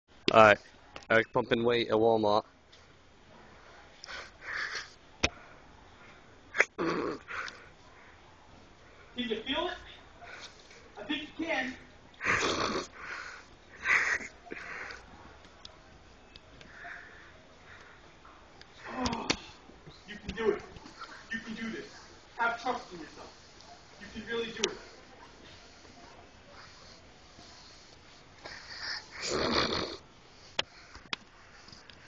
pumpin weight at walmart
pumpin weight and talkin to my self as people walk by lol